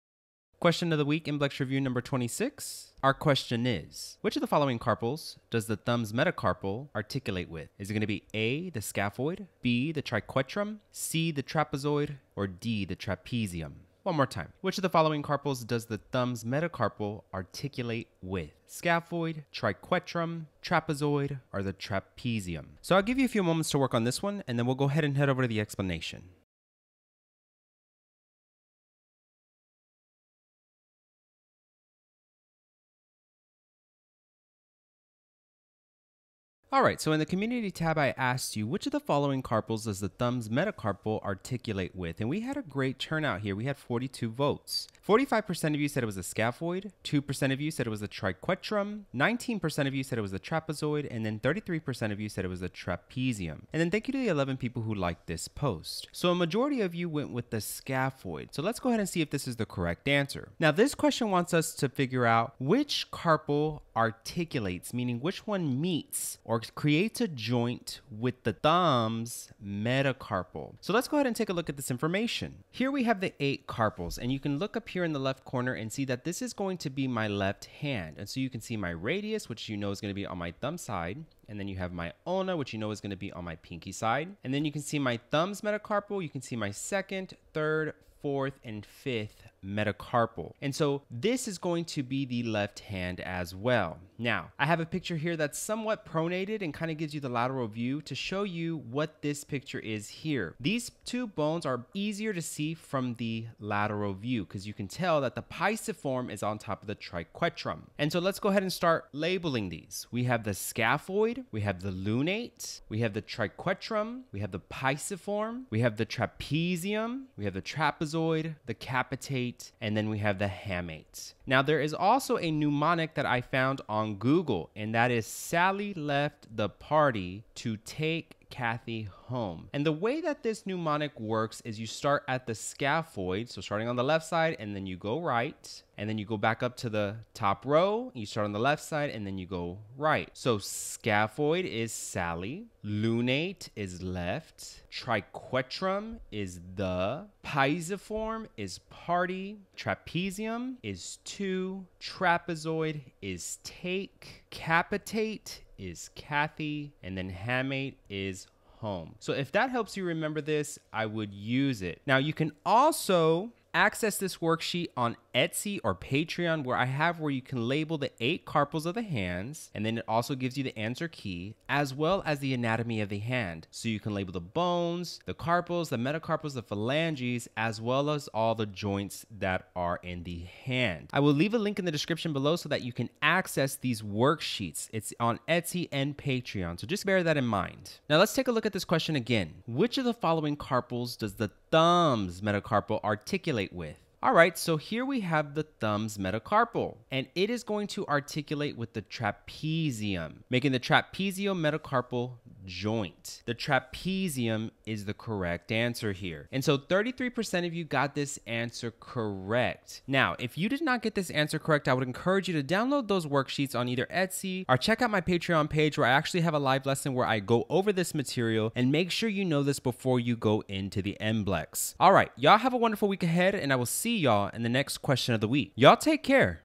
MBLEx Review: Question of the Week #26
This is the review question of the week...

Which of the following carpals does the thumb's metacarpal articulate with?

A) Schapoid

B) Triquetrum

C) Trapezoid

D) Trapezium

Make sure you know this information before going into the MBLEx.

You can find my email in the about me section of this YT channel.

To your success and very respectfully,
Well-Known Reyes